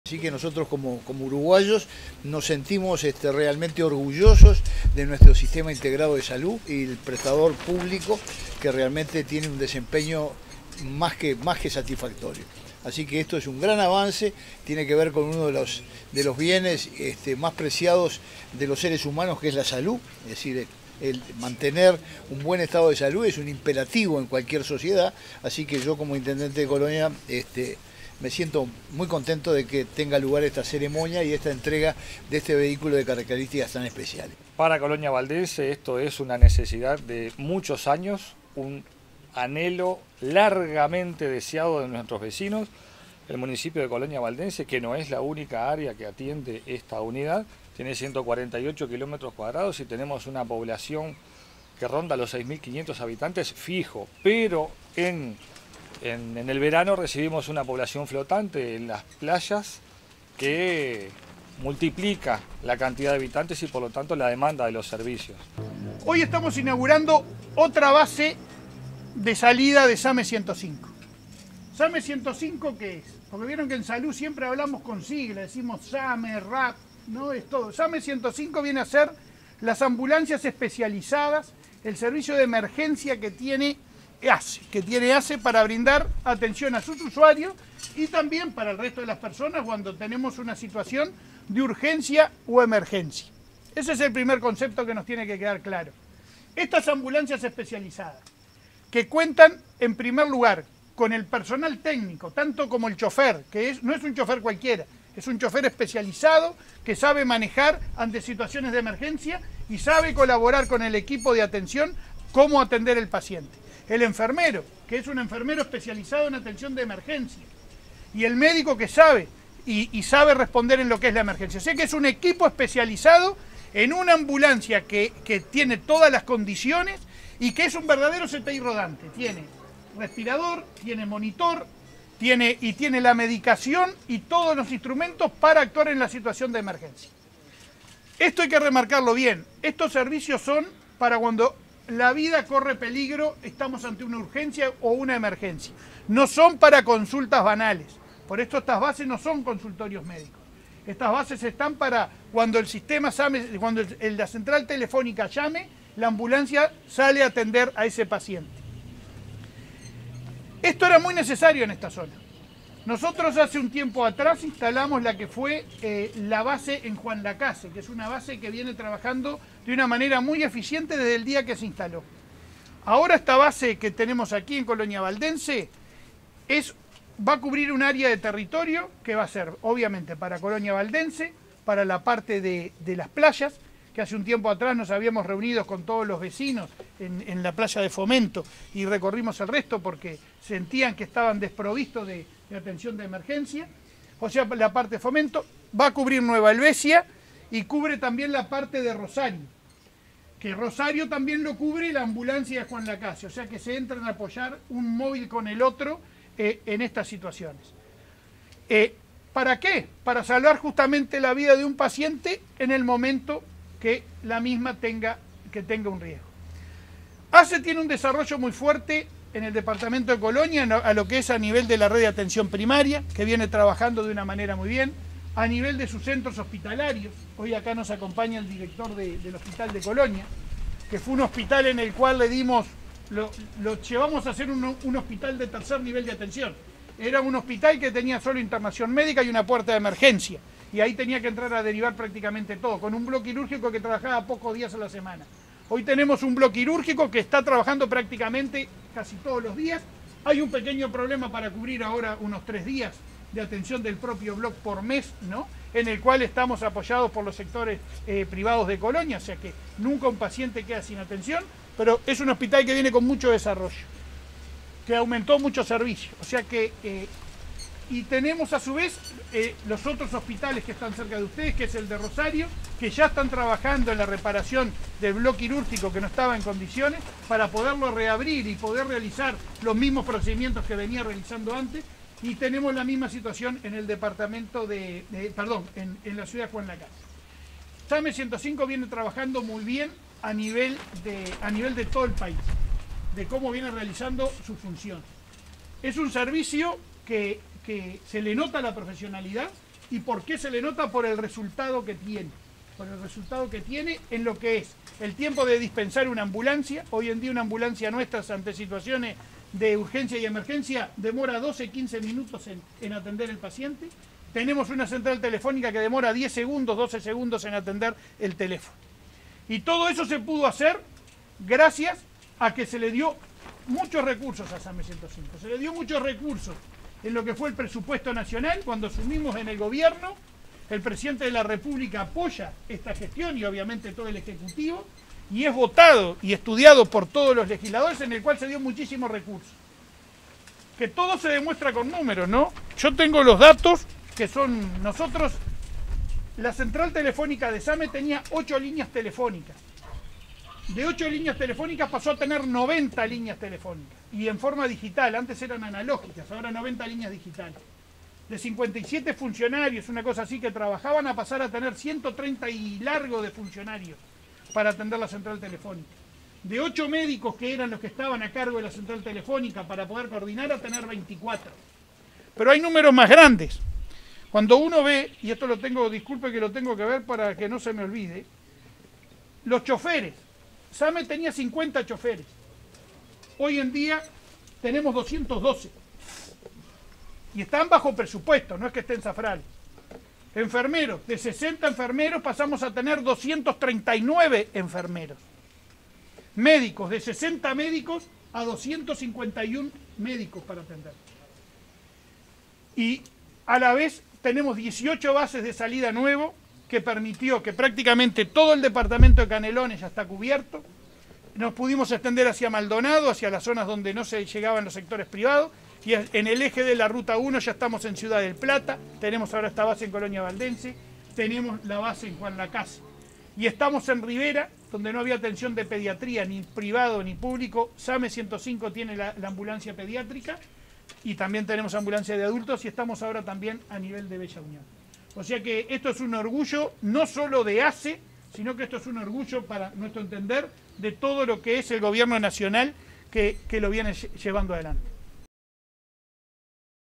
ASSE inauguró base del SAME 105 en Colonia Valdense
La localidad de Colonia Valdense vivió este martes 19 de diciembre un día histórico, con la instalación de una Base del Servicio SAME 105, con ambulancia especializada que es un verdadero CTI móvil, dotada de equipamiento y con recursos humanos médicos, de enfermería y chofer, para la atención de urgencias y emergencias.

El Alcalde Andrés Malán, sostuvo que la instalación de esta Base es de "justicia histórica", ya que concreta "un anhelo largamente deseado por nuestros vecinos". Mencionó que Colonia Valdense abarca una zona de 148 kilómetros cuadrados y tiene unos 6.500 habitantes estables, más una "población flotante que se triplica en verano" por la afluencia de público hacia los balnearios.

Por su parte, el Intendente de Colonia, Carlos Moreira, expresó que "como uruguayos nos sentimos orgullosos de nuestro Sistema Nacional Integrado de Salud y del prestador público [ASSE] que tiene un desempeño más que satisfactorio". Afirmó que esta Base, significa "un gran avance en lo que tiene que ver con uno de los bienes más preciados del ser humano que es la salud".

En tanto, el Presidente de ASSE, Leonardo Cipriani, expresó que la puesta en funcionamiento de este Servicio de Urgencia y Emergencia, forma parte del plan de descentralización llevado adelante por esta administración. En ese sentido, expresó que se trata de la Base número 18, que se apoyará en la ya existente en Juan Lacaze y brindará también cobertura a las localidades de Rosario, Nueva Helvecia y la zona balnearia que dista 11 kilómetros de Colonia Valdense. El jerarca aseveró que este crecimiento del Servicio SAME 105 se ha venido trabajando en los presupuestos nacionales. Al respecto, dijo que "lo primero fue mejorar la central telefónica", pasando de 8 líneas analógicas a 90 líneas digitales; de 57 funcionarios a más de 130 y de 8 médicos coordinadores a 24. Esto permitió una atención telefónica rápida. En ese aspecto, recordó que "años atrás, las grandes quejas era el tiempo de demora en la atención telefónica, que llegaba a 40 minutos: ahora es de 12 segundos".

Asimismo, destacó que se pasó de 52 a 212 choferes, de 60 enfermeros a 239 y de 60 a 251 médicos. Agregó que se incrementó notablemente el número de ambulancias y que el Servicio, restringido a Montevideo, se extendió a Ciudad del Plata en San José, a prácticamente todo el departamento de Canelones, a La Capuera en Maldonado, a Rivera y Bella Unión. "Estamos orgullosos de todo el equipo del SAME", enfatizó Cipriani. Por otra parte, el Presidente de ASSE señaló los avances realizados durante esta gestión en el Hospital de Colonia, cuyo CTI cuenta con "indicadores médicos a la altura de cualquiera de los grandes hospitales de Montevideo". Mencionó que en este momento, se está trabajando para la instalación del tomógrafo.

Añadió que el 31 de octubre cerró la licitación y ahora se está en el proceso de adjudicación. Además de las autoridades ya nombradas, participaron del evento el Vicepresidente de ASSE, Marcelo Sosa, el Vocal, Julio Micak, la Directora en Representación de los Usuarios, Natalia Pereyra, el Coordinador Subregional, Aldo Silveira, autoridades del SAME 105, Alcaldes de otras localidades del departamento de Colonia, legisladores, concejales, representantes de diferentes instituciones y organismos, funcionarios y usuarios.